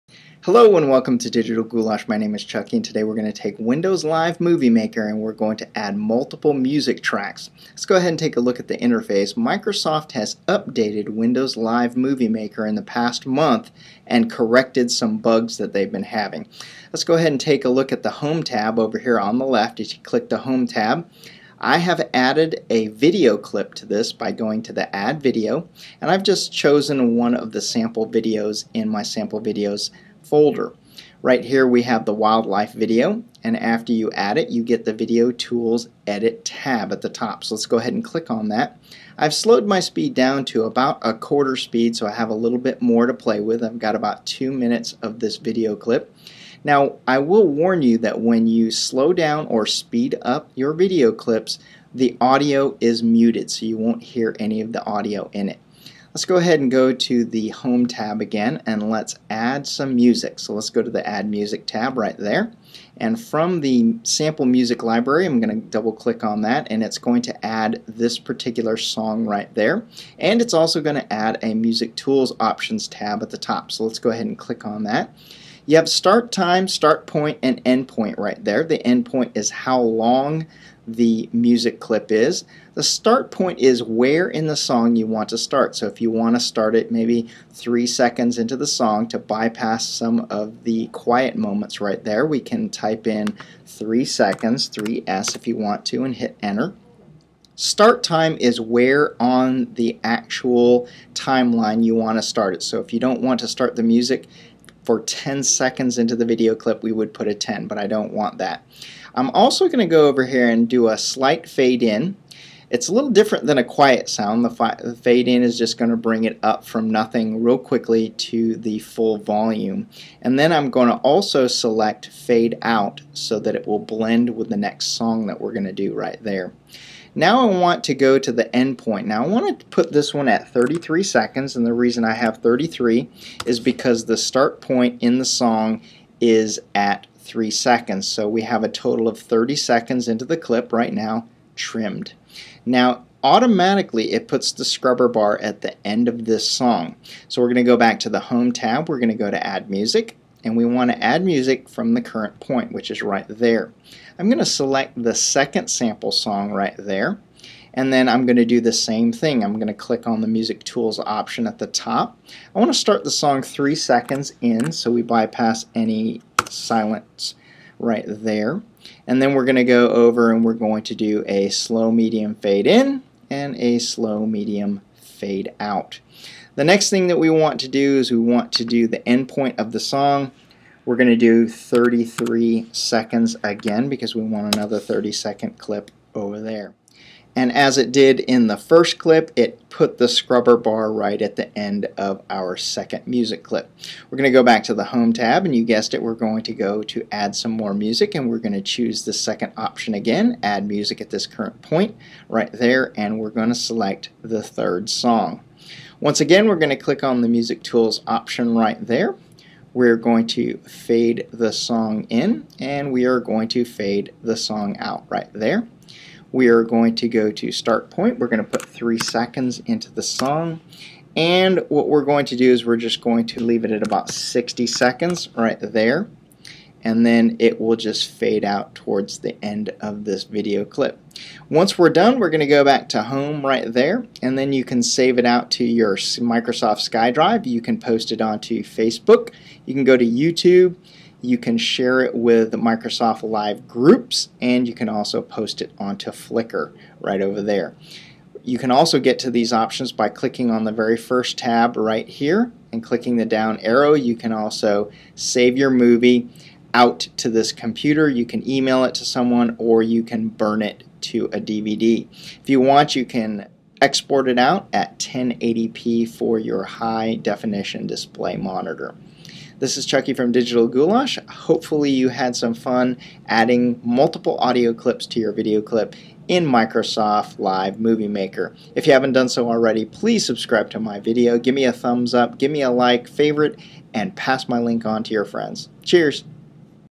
Windows Movie Maker Made Easy - Add Multiple Music Clips to Video
This video demonstrates how to add, fade, cut, and mix music tracks to video.

Check out the new built-in version of Windows 10 Video Editor by checking out my video